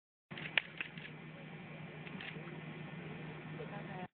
LSU Football Handshakes
Junior defensive tackles Ego Ferguson and Anthony Johnson practice their handshake during player interviews on Monday. Each position group designs a handshake to be performed during games.

Video by Trey Labat